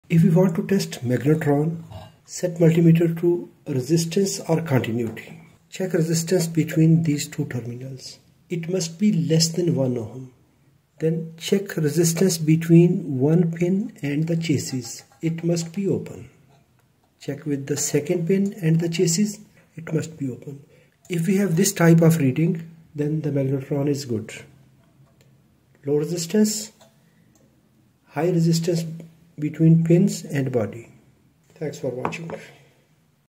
how to test magnetron
how to test magnetron magnetron is the major component in microwave o to oven, it receives 3.1 to 3.2 vac and 415KV (4.1 to 4.25KV) dc voltage. and it provides 2458 MHz (2448 to 2468 MHz) frequency beam is produced by magnetron. it can be checked by multimeter.